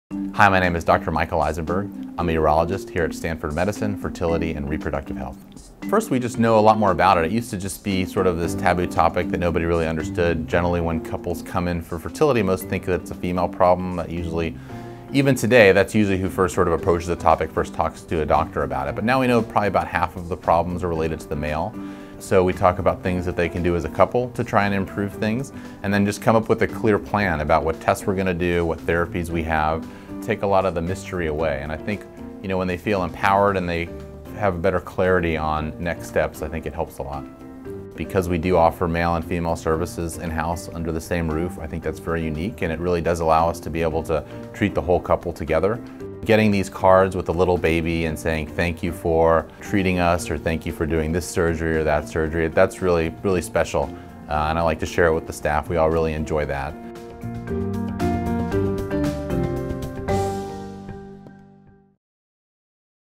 Michael Eisenberg, MD – Fertility and Reproductive Health, Stanford Medicine
Michael Eisenberg, MD, describes his work as a urologist at Stanford Medicine Fertility and Reproductive Health. He specializes in fertility treatment for men.